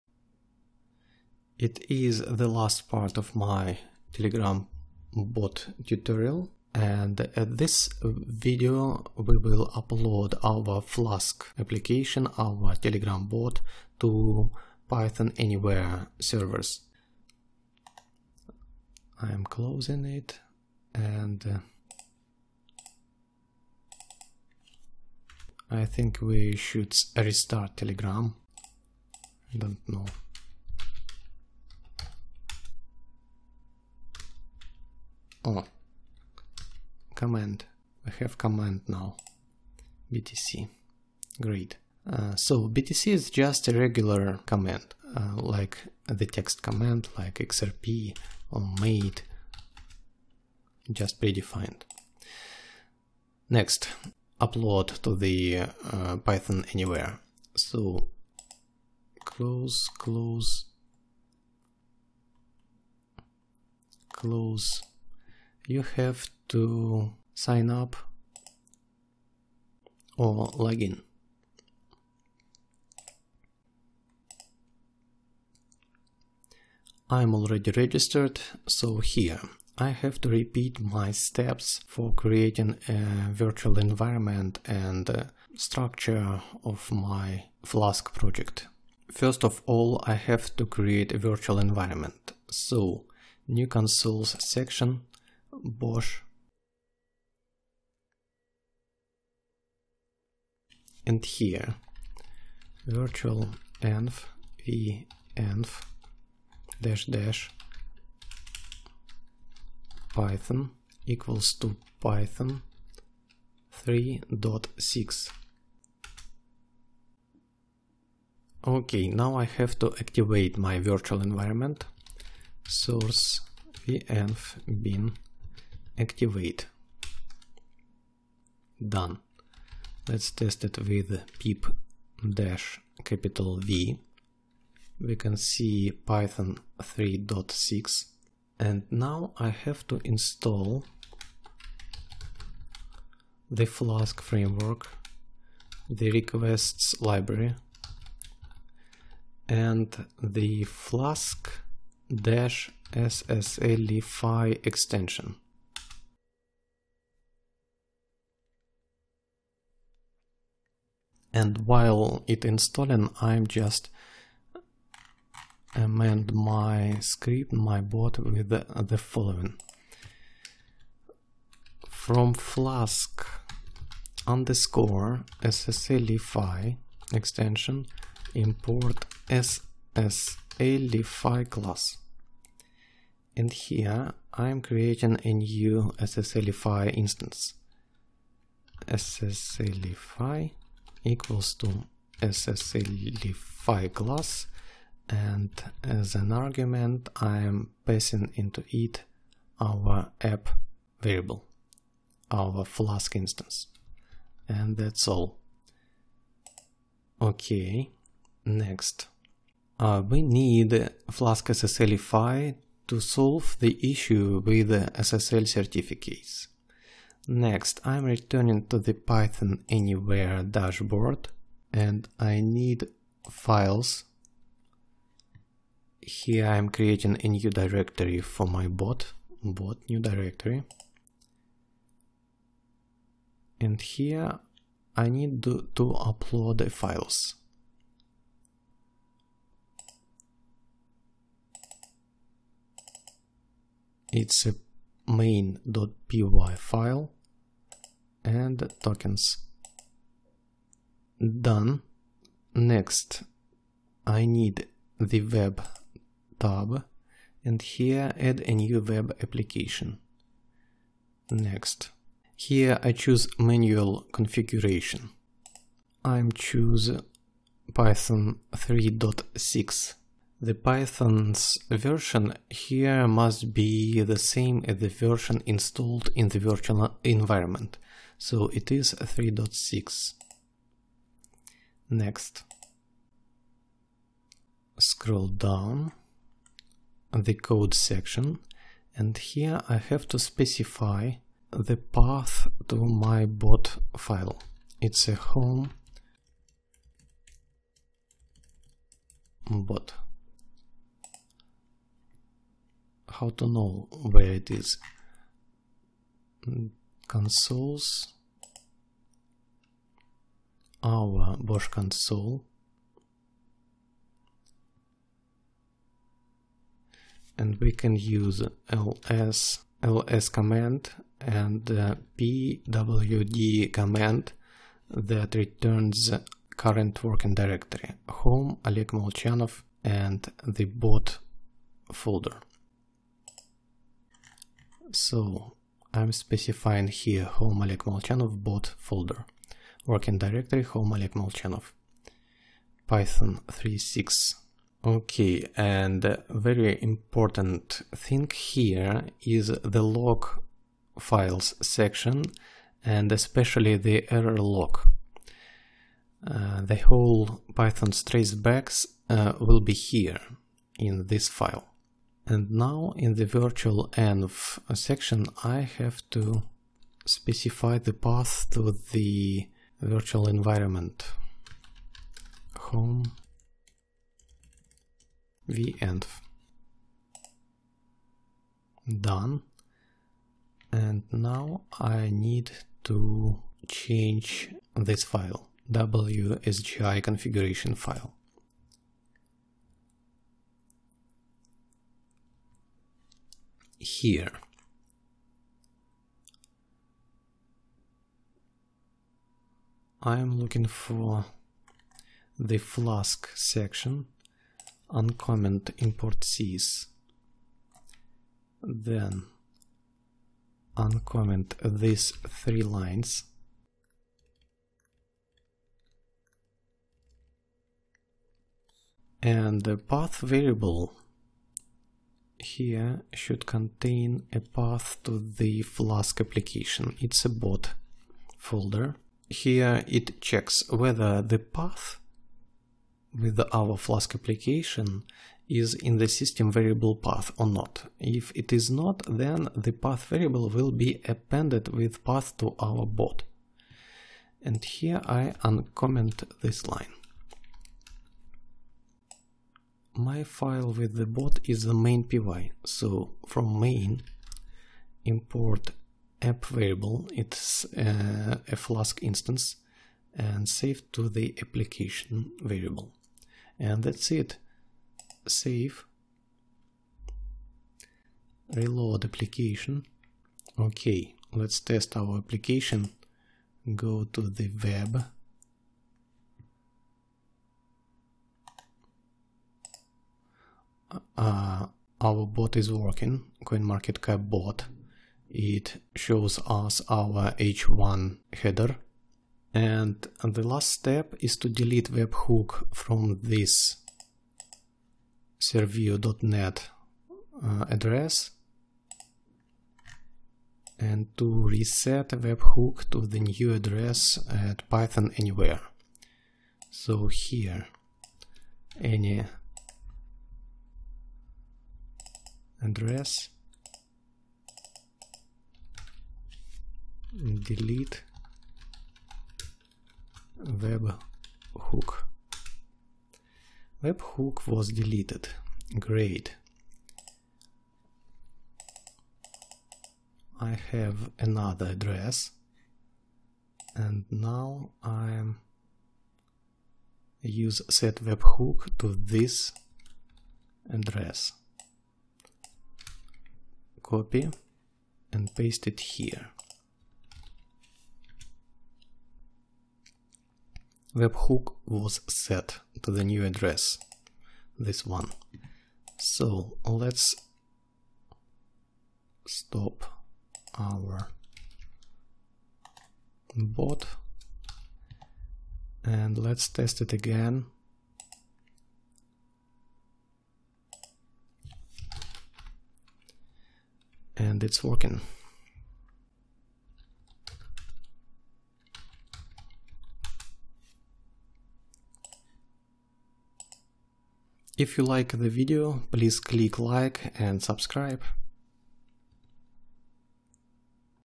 Python Telegram Bot Tutorial: How to deploy to PythonAnyWhere #4 | Python project
In this part #4 of Python Telegram Bot Tutorial I'll show hot to deploy the Telegram Bot to the PythonAnyWhere hosting.

The Telegram Bot takes a command (cryptocurrency ticker) from a user, such as: /btc, /eth, /xrp, etc., performs a request to the CoinMarketCap API and returns to the user a price of the cryptocurrency in USD.

The Telegram Bot is a Flask framework application that uses a webhook to receive updates.

- # 3. Creating bot, webhook, testing

Covered topics:

1. Dealing with CoinMarketCap API
2. Making GET and POST requests with the Requests library
3. Dealing with json-objects with Python (reading and writing)
4. Creating a Flask application. Handling POST-requests with a Flask
5. Searching a text with regular expressions
6. Creating a Telegram Bot with BotFather. Adding a bot commands.
7. Dealing with the Telegram bots API.
8. Testing a webhook with tunneling services
9. Deploy Telegram bot (a Flask application) to the PythonAnyWhere servers.
10. Solving a SSL certificates issue with the Flask-SSLify extension